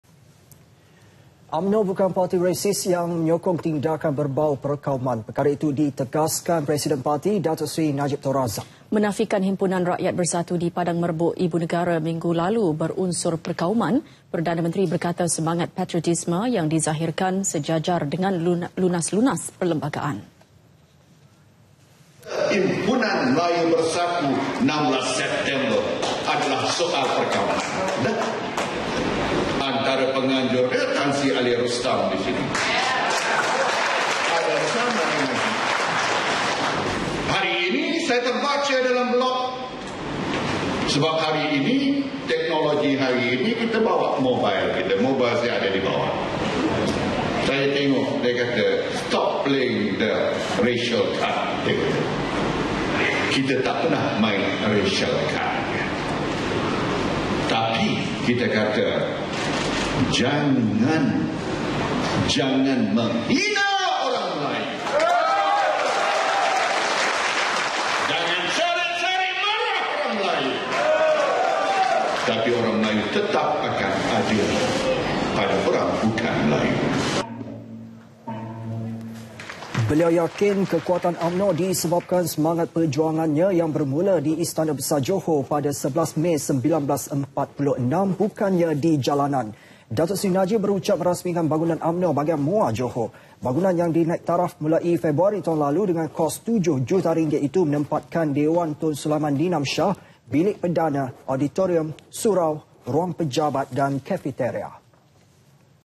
UMNO- UMNO BUKAN RASIS- PM [20 SEPT 2015]
UMNO bukan parti rasis yang menyokong tindakan berbaur perkauman. Perkara itu ditegaskan Presiden parti, Datuk Seri Najib Tun Razak.

Menafikan 'Himpunan Rakyat Bersatu' di Padang Merbok ibu negara minggu lalu berunsur perkauman, Perdana Menteri berkata... semangat patriotisme yang dizahirkan sejajar dengan lunas-lunas perlembagaan.